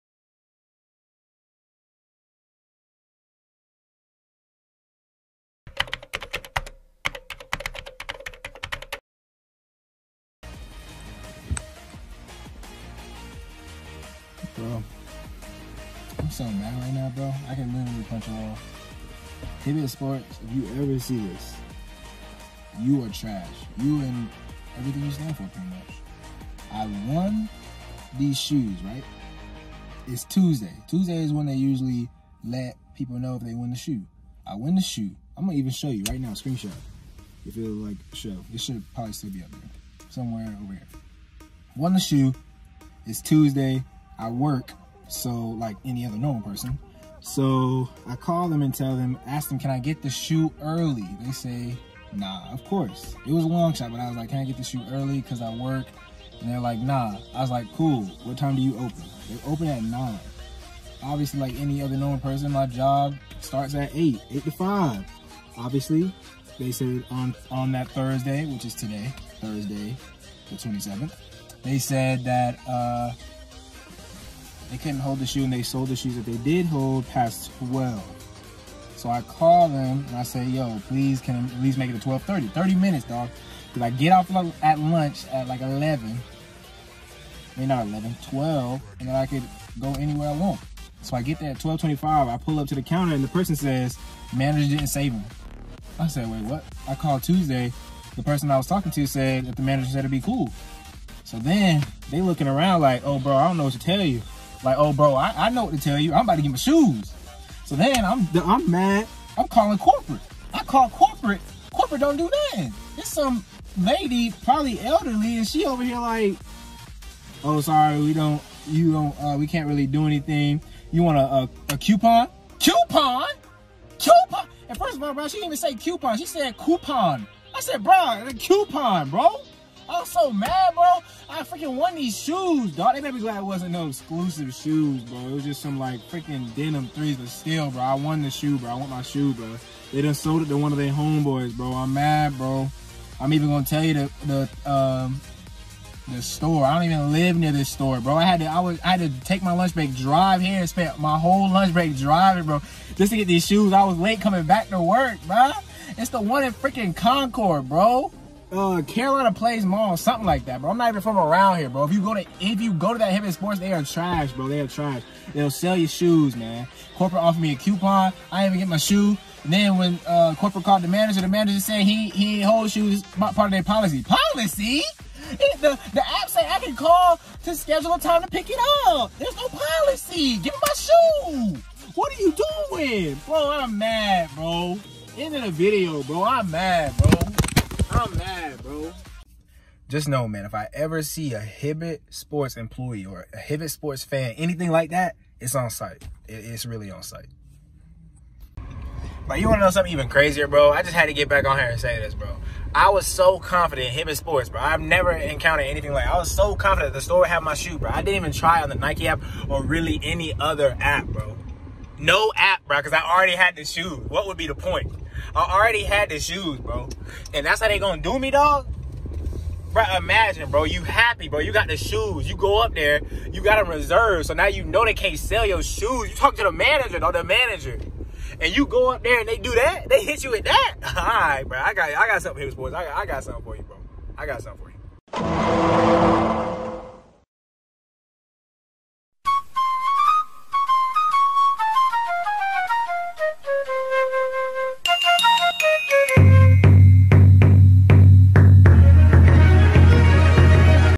Hibbett Sports FINESSED Me (EMPLOYEE STOLE FROM ME!!!)
Mathew 6:33 - Seek the Kingdom of God above all else, and live righteously, and he will give you everything you need.

P.S.